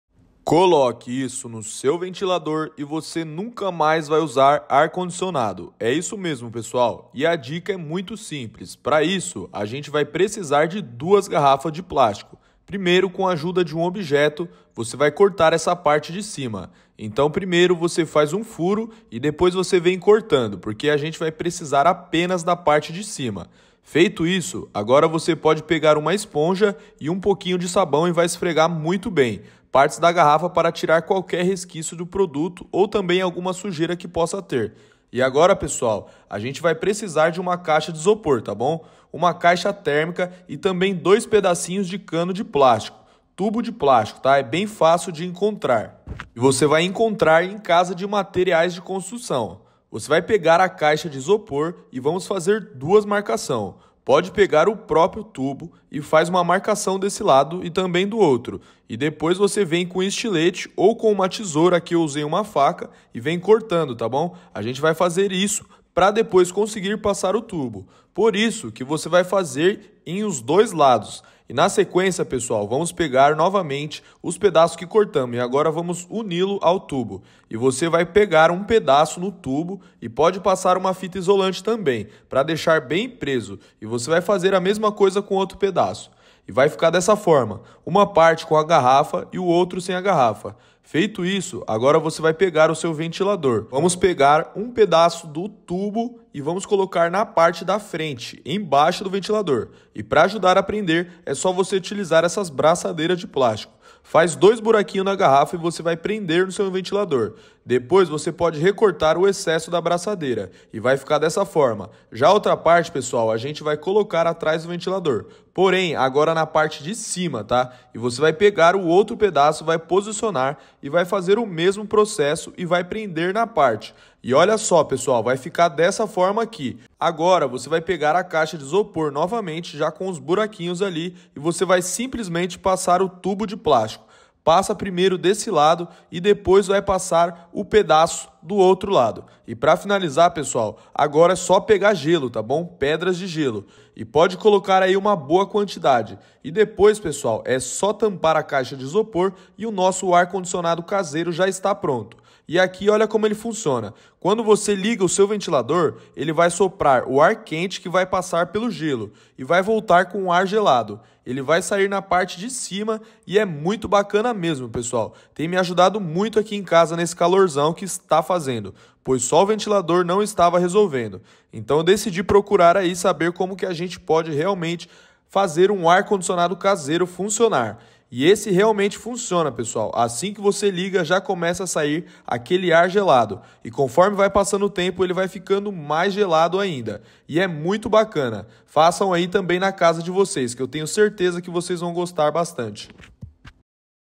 Ar Condicionado Caseiro!! Fácil e pratico sem gastar Energia
Faça seu próprio ar condicionado caseiro na sua casa sem gastar energia, Muito econômico e com coisas que você tem em casa